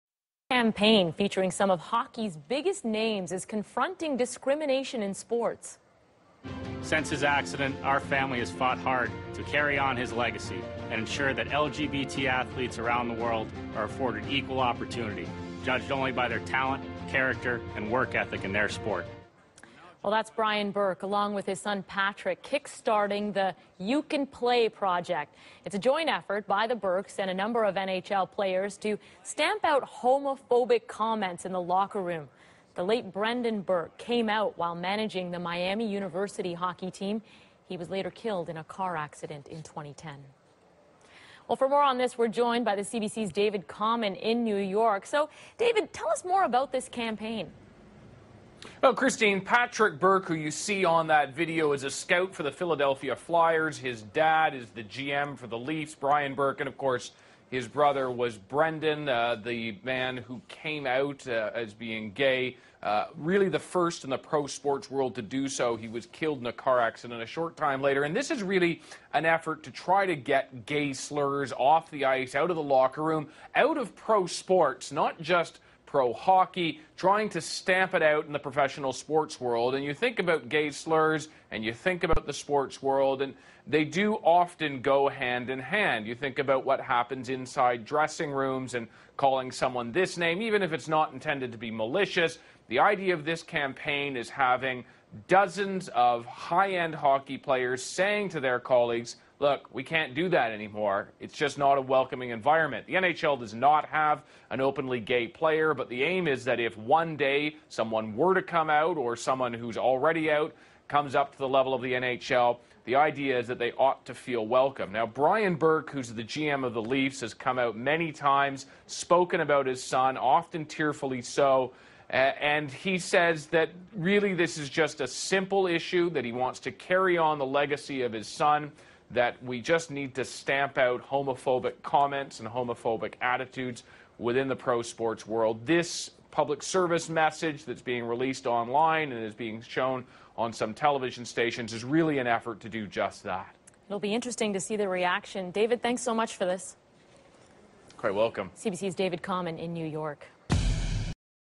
You can play - expanded.flv
CBC television story about the tolerance campaign called "you can play", intended to fight homophobia in the NHL and professional hockey generally.